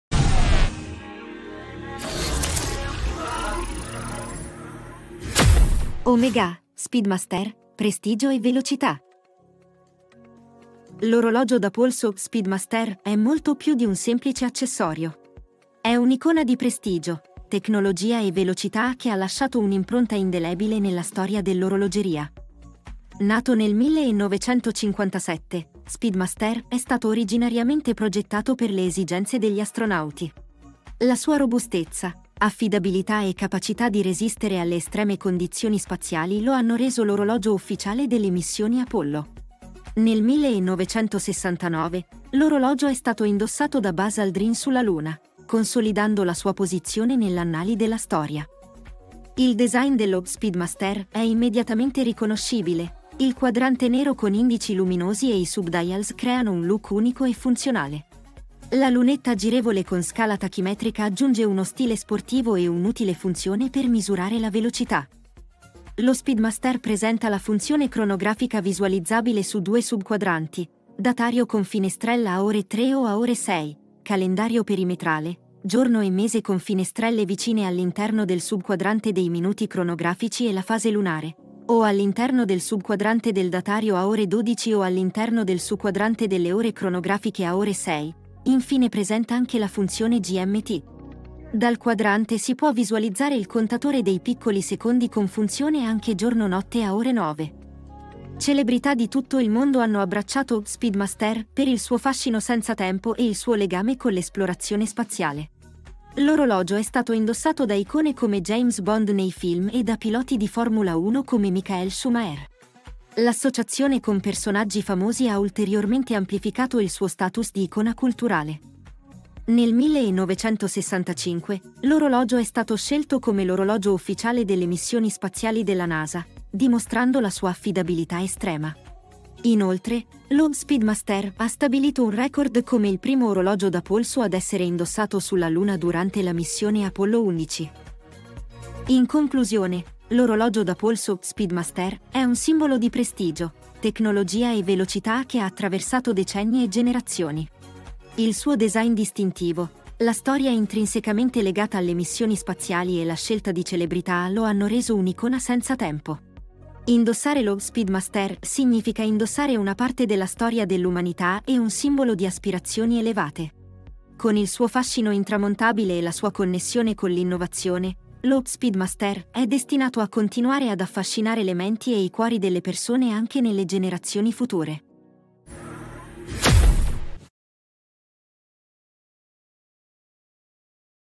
OMEGA SPEEDMASTER : Il cronografo che ha conquistato la Luna e il mondo
Scopri perché l’Omega Speedmaster è molto più di un orologio, ma un simbolo di velocità, innovazione e avventura, che ha ispirato generazioni di sognatori.